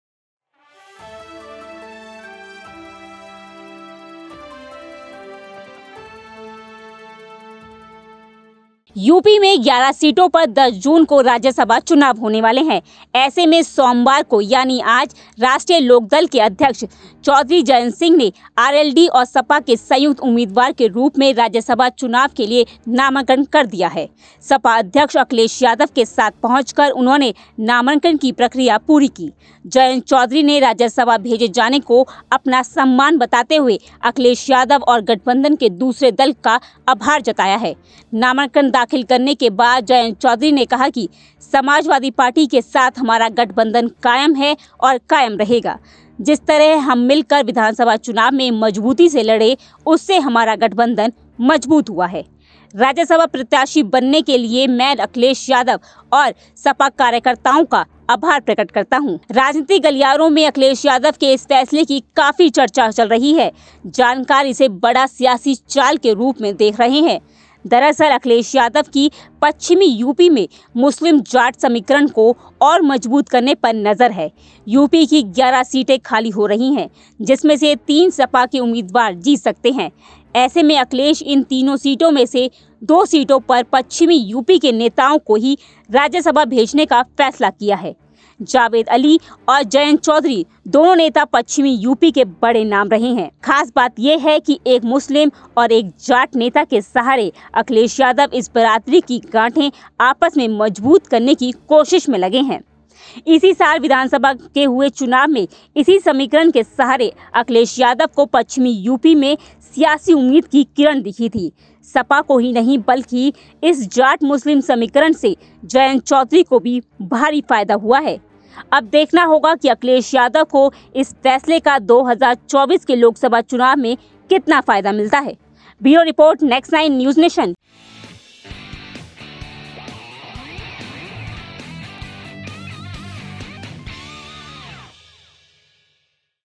Rajya Sabha Election 2022: Jayant Chaudhary ने दाखिल किया नामांकन, Akhilesh ने खेला दांव !
10 जून को यूपी में राज्यसभा चुनाव
जंयत चौधरी ने दाखिल किया नामांकन
अखिलेश ने उठाया अहम सियासी कदम
अखिलेश की पश्चिमी यूपी पर नजर !
यूपी में 11 राज्यसभा सीटों पर होना है चुनाव
जंयत चौधरी ने RLD-सपा से दाखिल किया नामांकन
जयंत ने गठबंधन कायम रखने का किया दावा
सम्मान देने के लिए अखिलेश यादव का धन्यवाद-जयंत